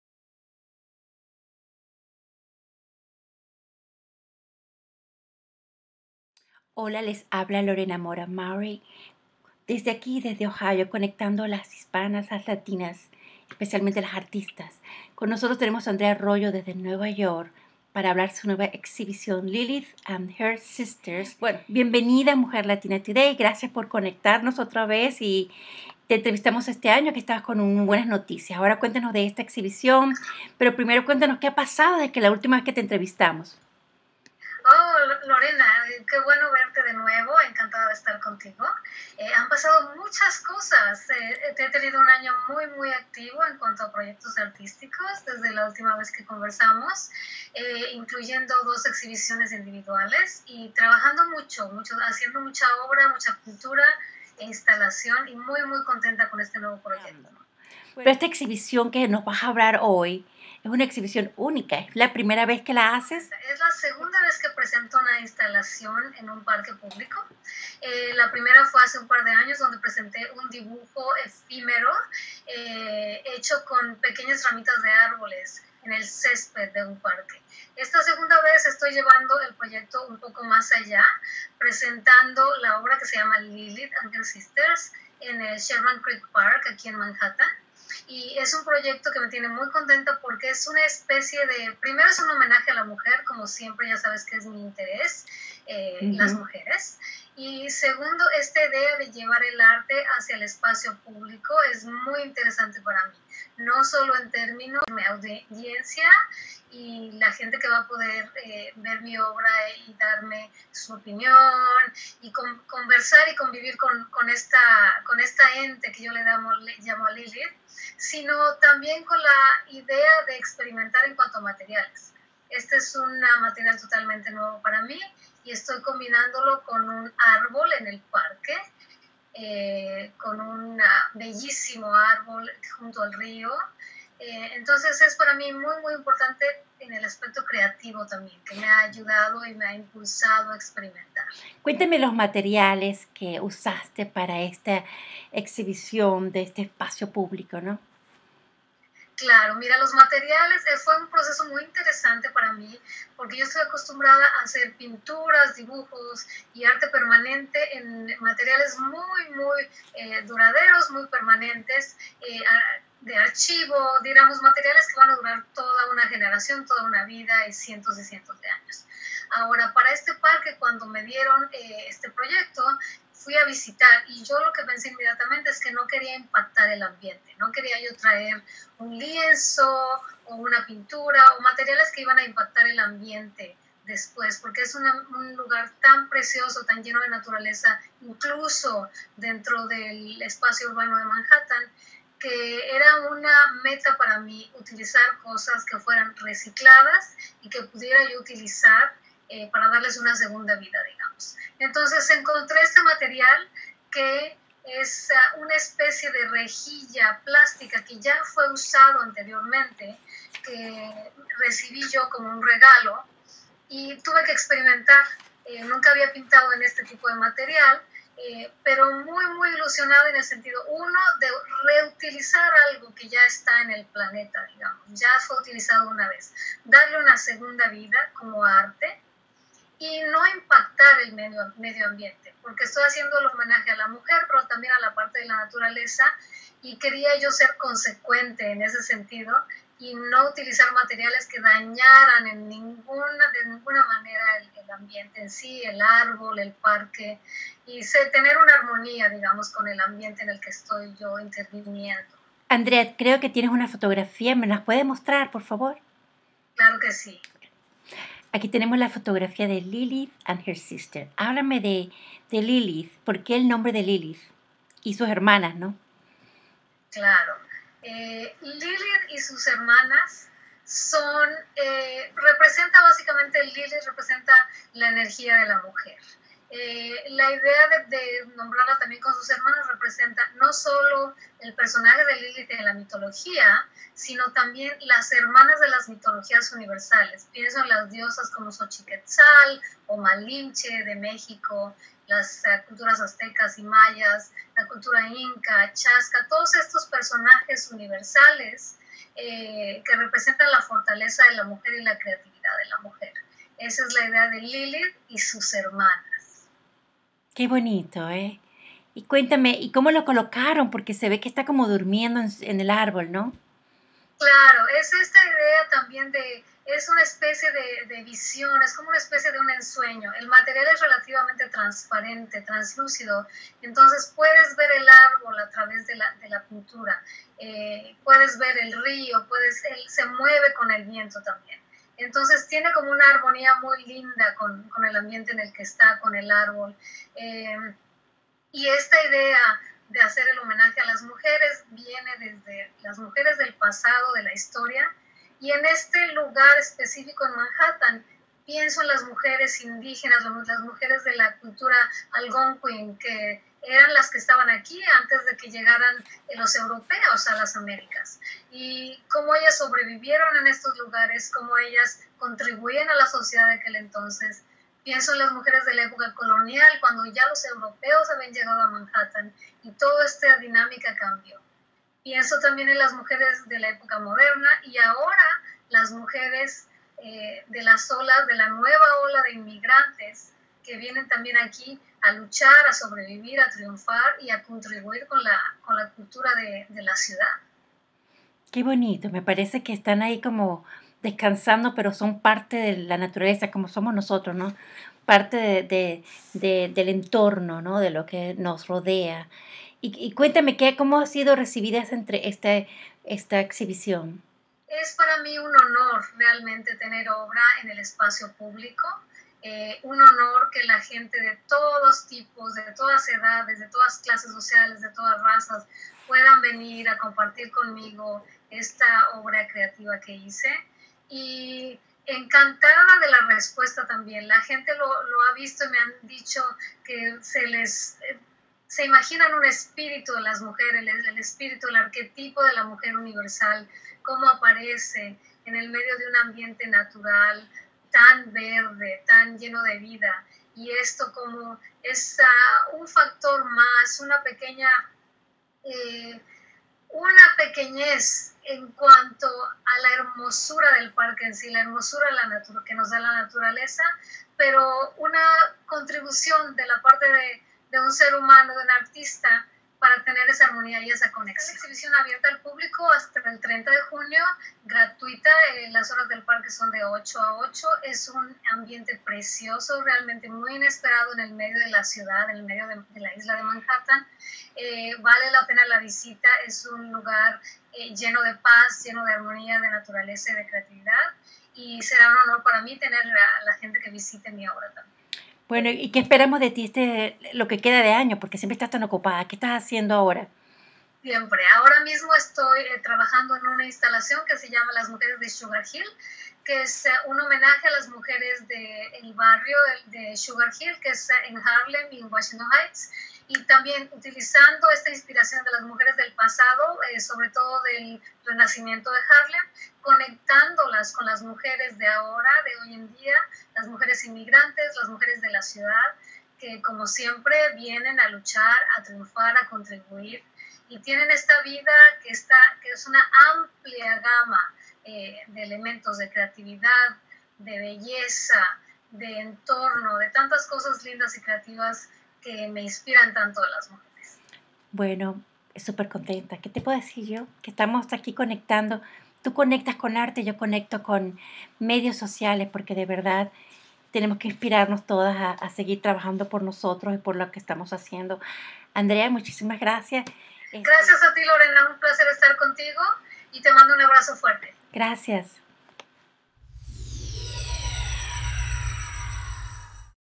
Andrea Arroyo: "Lilith and her Sisters" - Homenaje a la Mujer Universal
La artista plástica mexicana Andrea Arroyo es entrevistada por Lorena Mora-Mowry para hablar sobre su último proyecto: "Lilith and her Sisters©". Estas piezas de arte fueron comisionadas por el NoMAA y the New York Restoration Project para ser expuestas en en el Parque Sherman Creek, New York. "Lilith and her Sisters©" es un homenaje a la mujer universal y la pueden ver hasta el 30 de junio 2014.